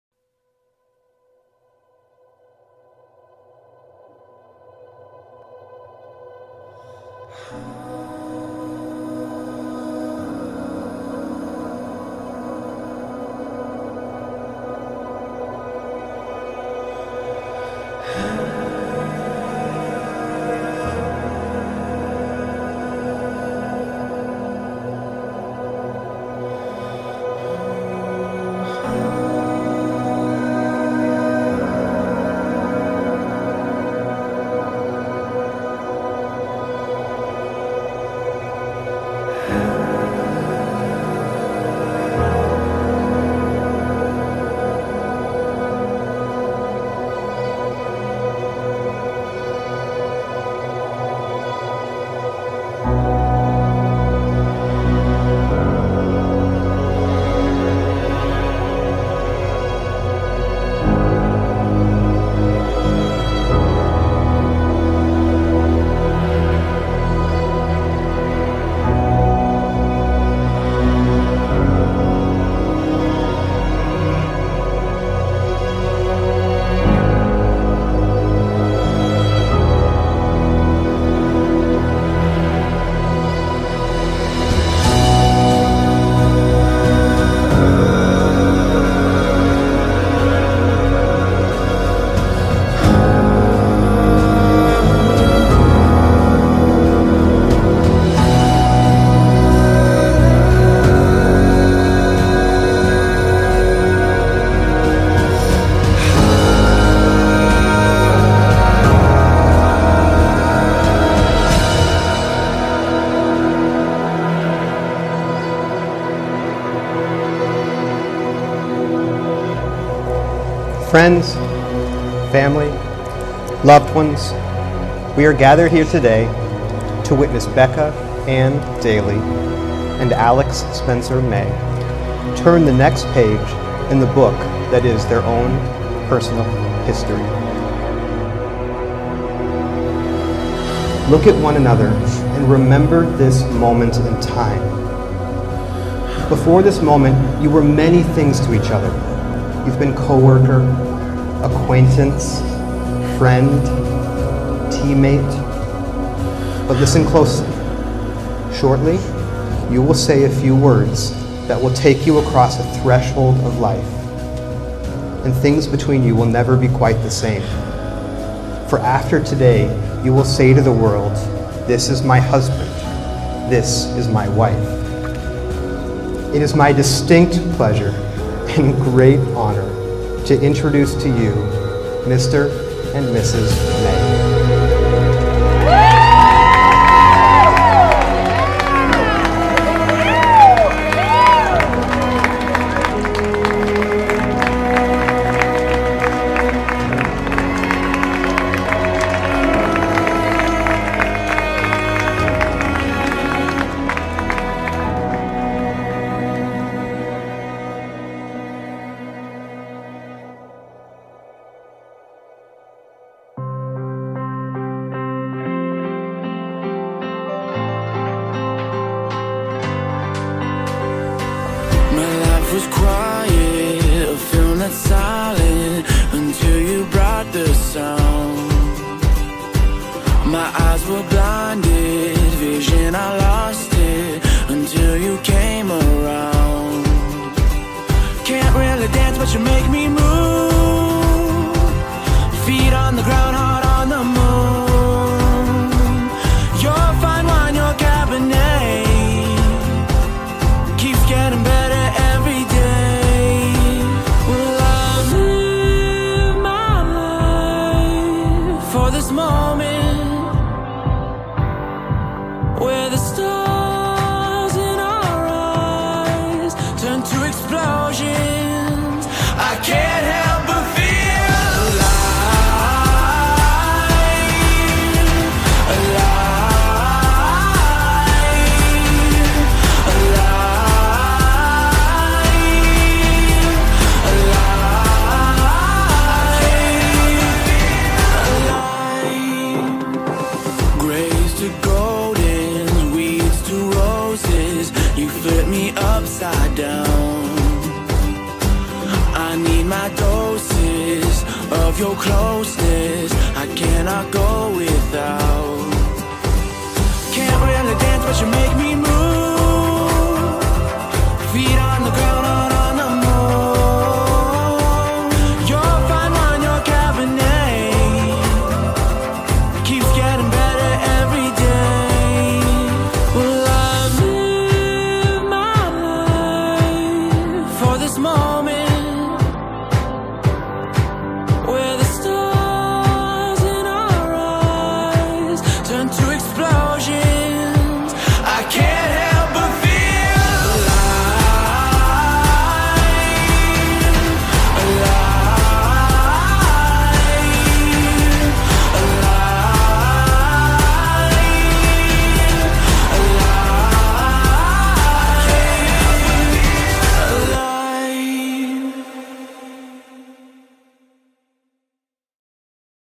Villa Siena - Gilbert, AZ - Alex + Becca Highlight Film
Congratulations to this amazing couple!!

Venue - Villa Siena in Gilbert, AZ

Film Produced By: Everlasting Cinema